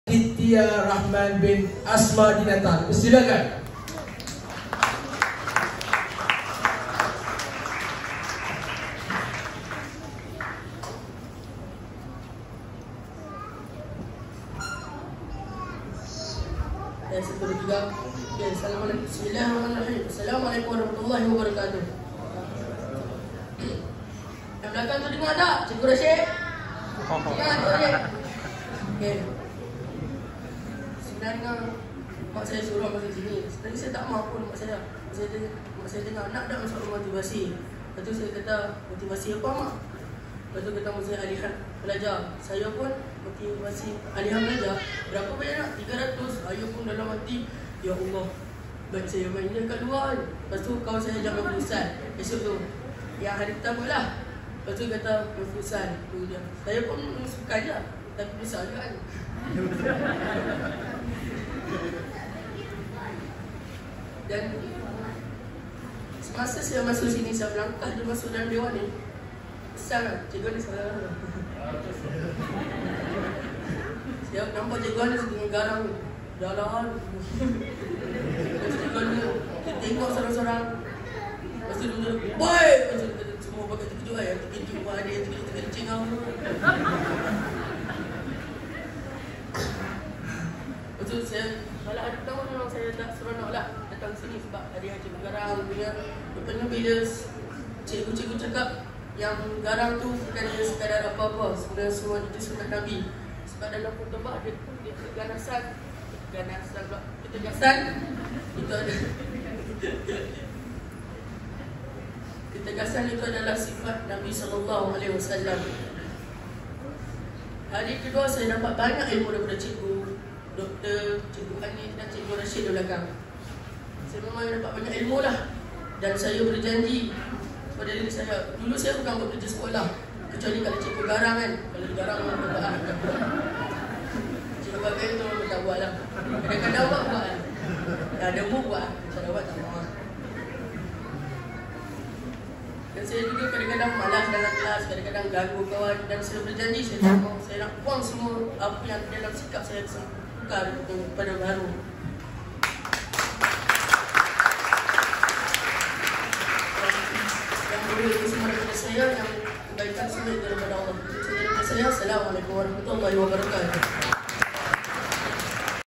Ucapan Peserta Keempat di Majlis Penutup Kursus MAP di Alor Setar
Untuk info lanjut tentang Kursus Motivasi Alihan Pelajar yang diterajui oleh Dr. Hj. Tengku Asmadi, sila layari